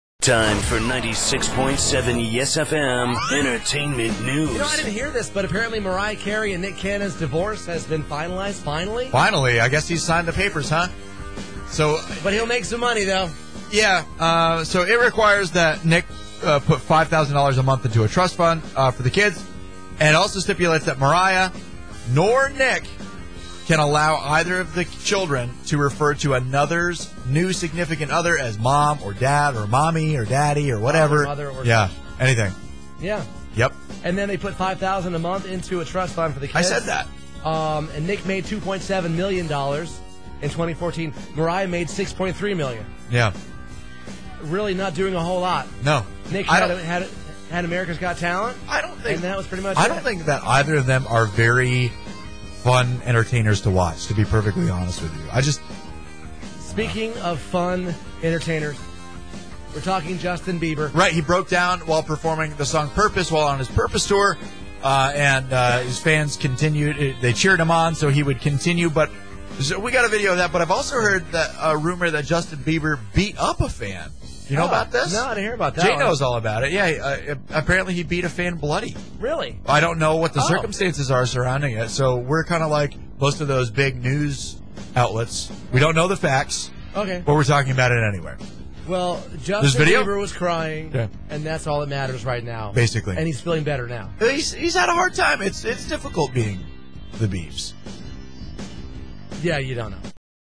YES-FM Entertainment News for Thursday 11-24-16
Nick and Mariah divorce deets

Justin Bieber breaks down